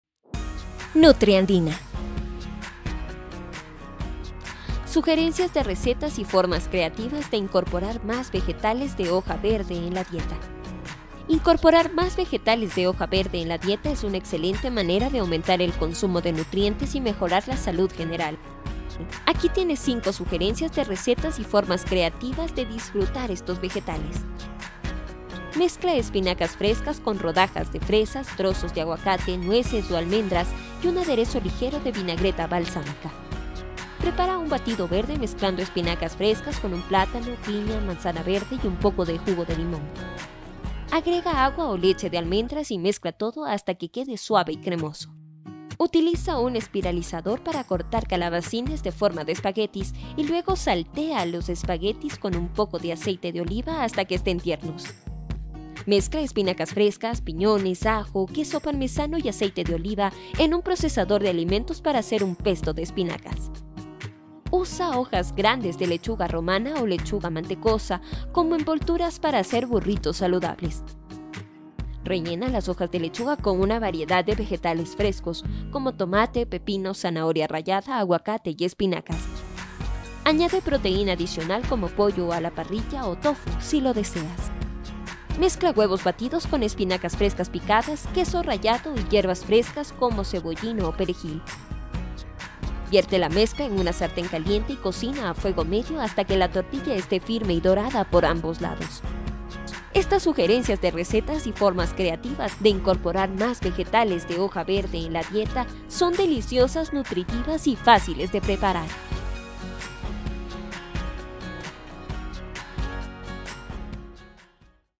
NutriAndina - 5 sugerencias de recetas de incorporar más vegetales de hoja verde en la dieta
Incorporar más vegetales de hoja verde en la dieta es una excelente manera de aumentar el consumo de nutrientes y mejorar la salud en general. Aquí tienes cinco sugerencias de recetas y formas creativas de disfrutar estos vegetales.